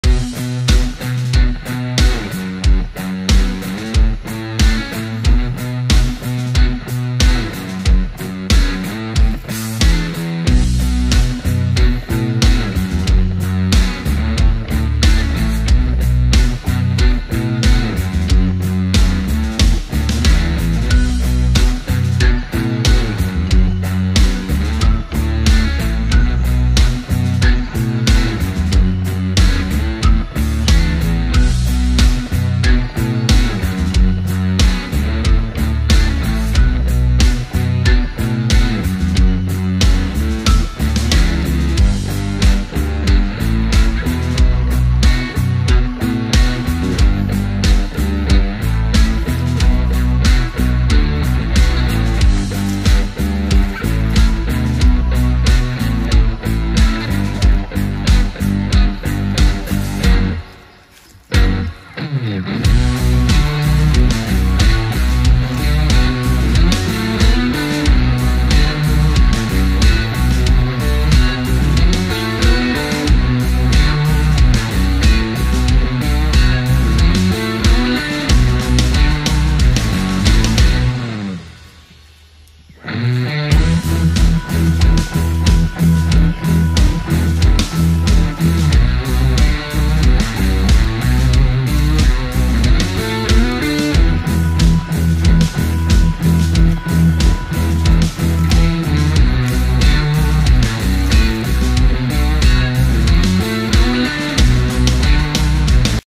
United States vs Wales, who will win? Let's predict the world cup 2022
Let's predict the US vs Wales football team with the rabbit to see who will win the football match of the 2022 World Cup